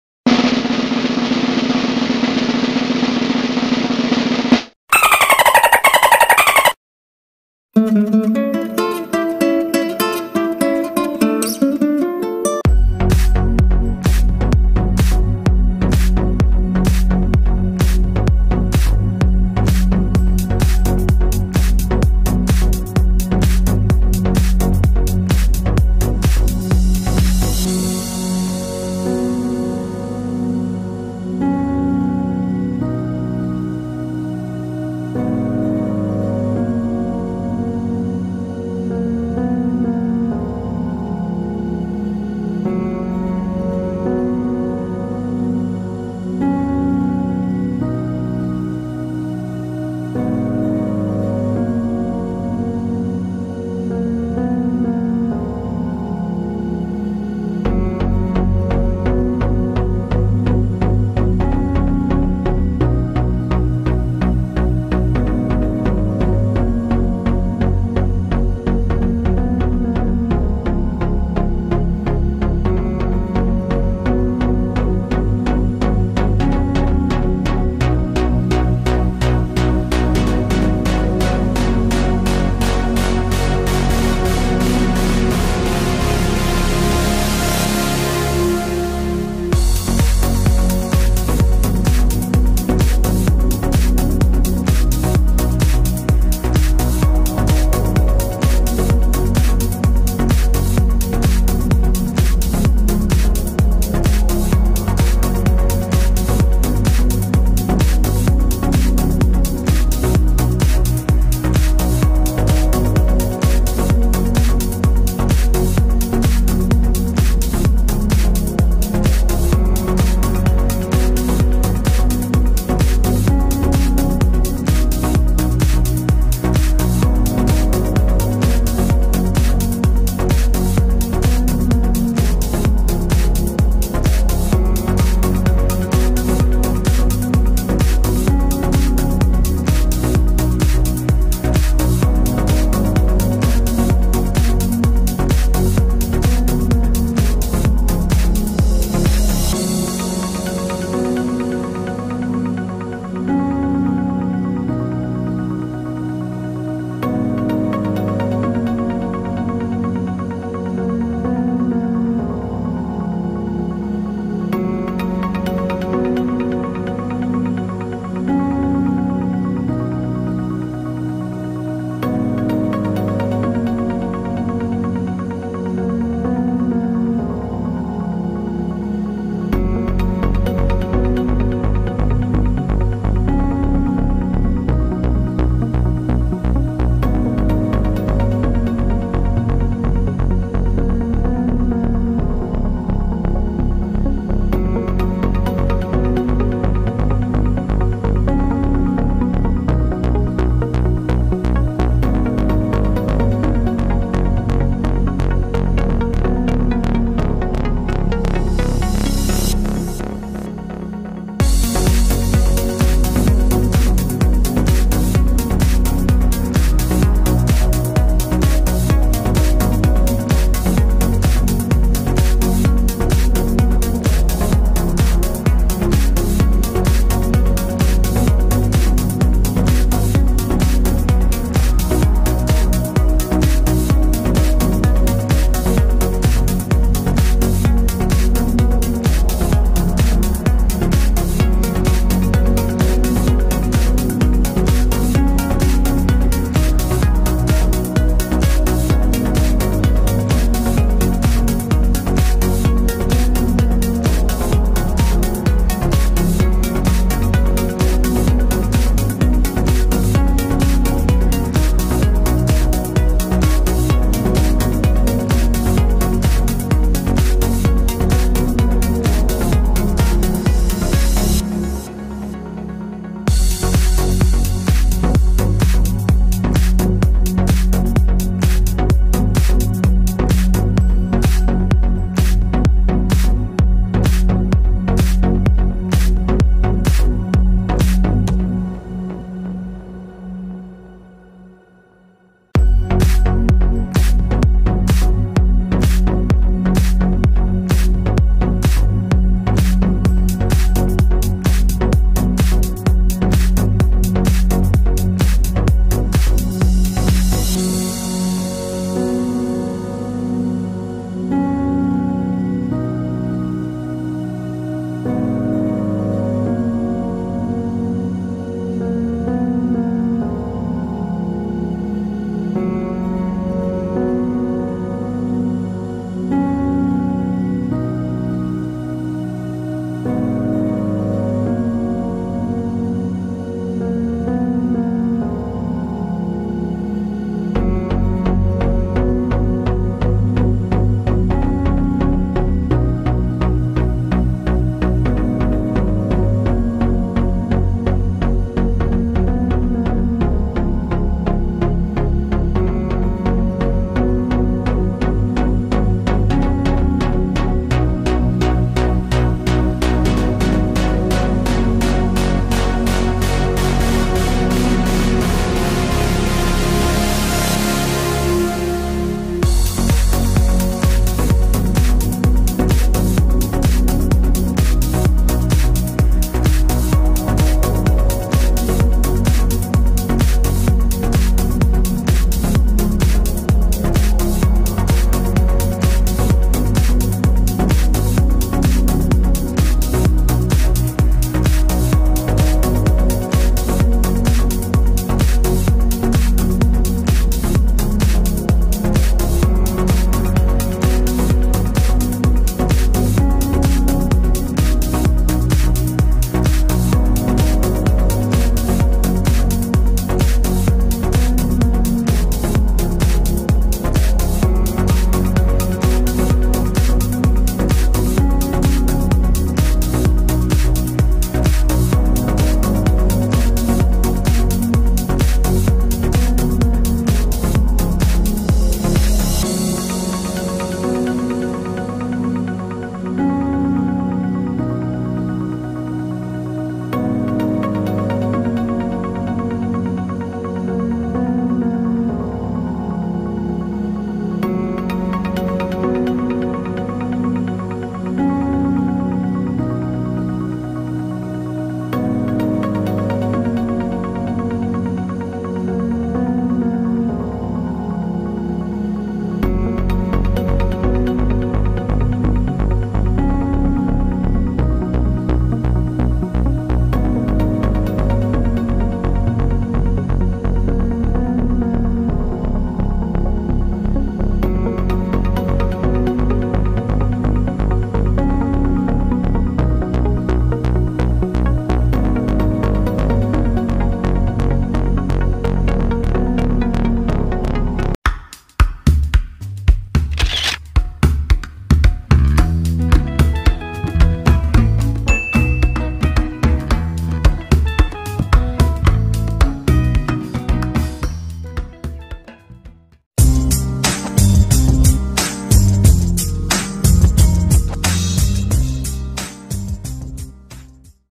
Sea Lion Show | Manila Ocean Park 2019
watch the amazing creatures of the sea perform
the sea lion show at manila ocean park
they are trained and taken good care
Equipment Used:
Zhiyun Smooth-Q Handheld Gimbal Stabilizer for Smartphone
Fotopro uFO2 Flexible Tripod